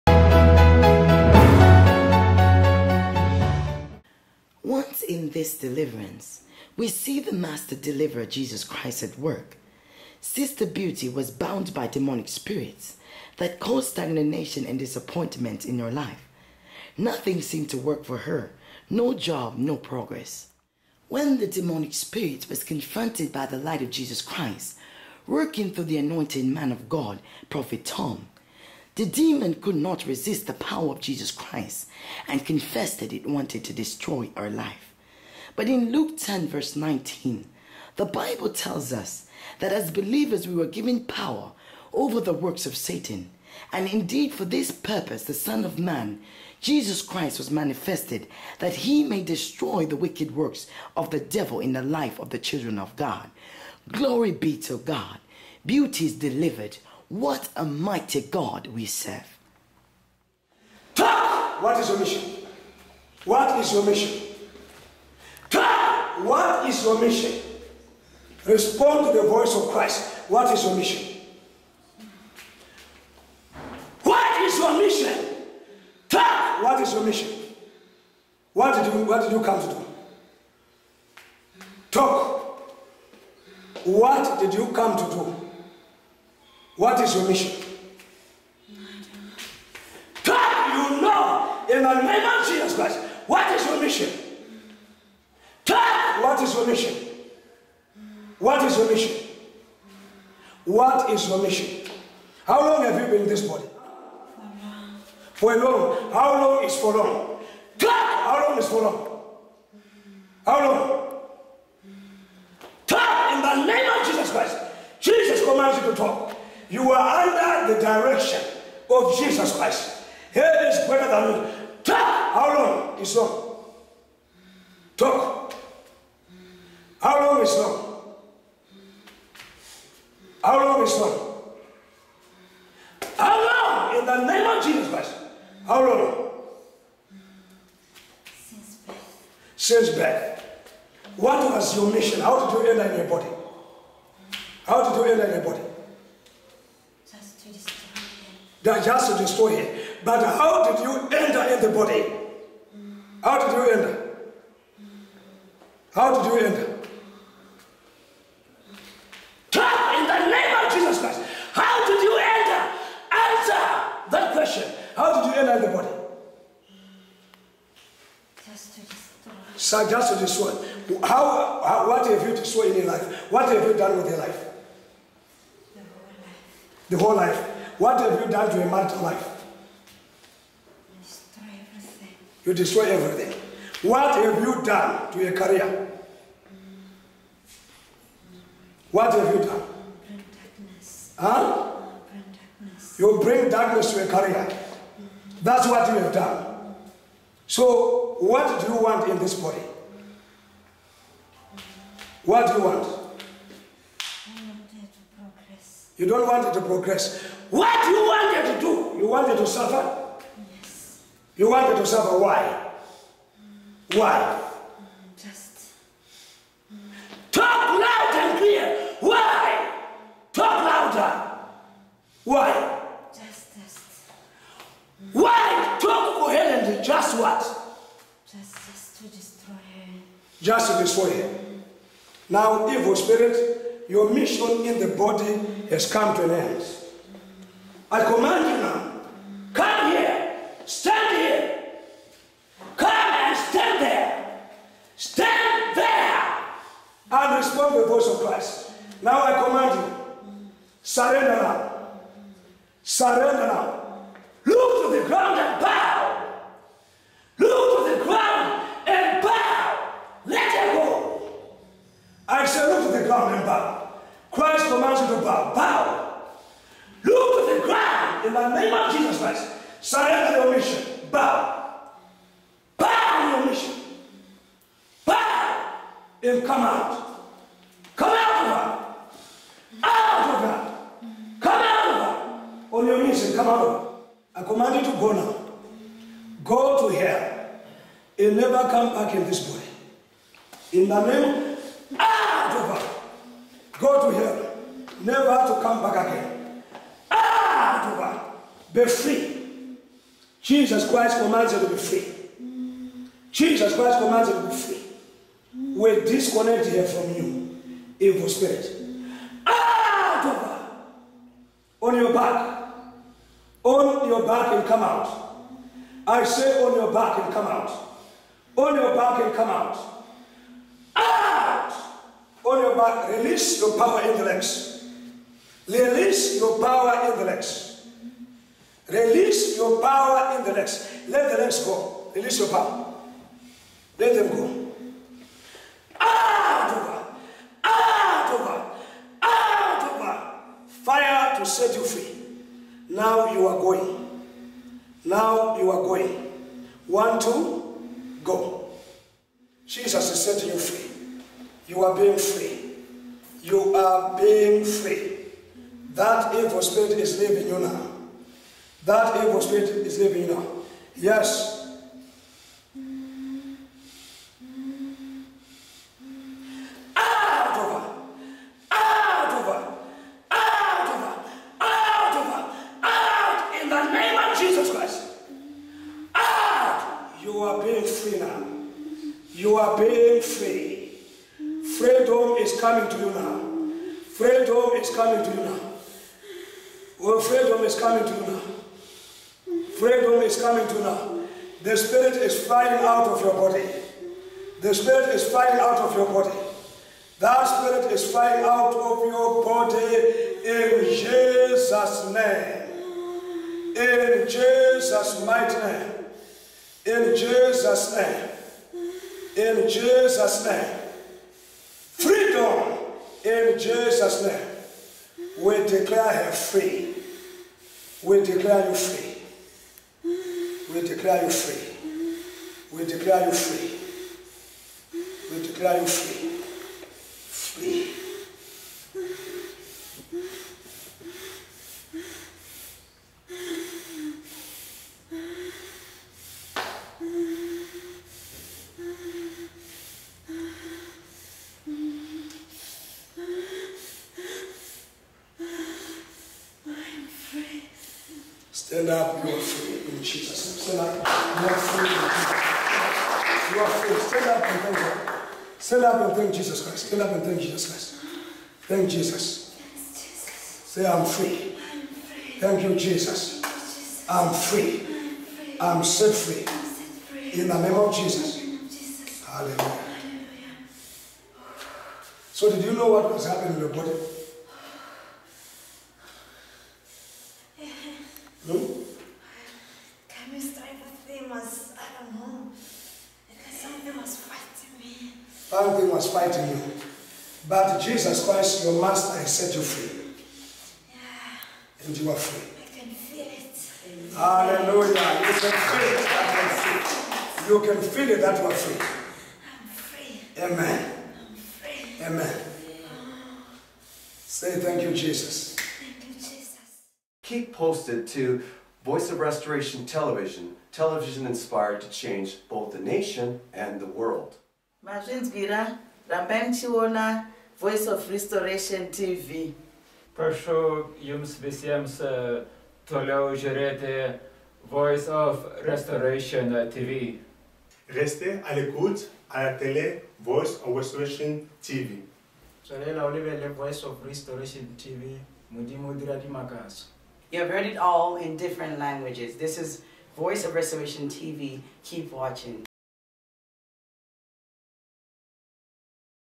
Deliverance from a Spirit of Lucifer(Satan)
God Almighty is still delivering his people. Satan appears outwardly strong but in the inside, he has no power.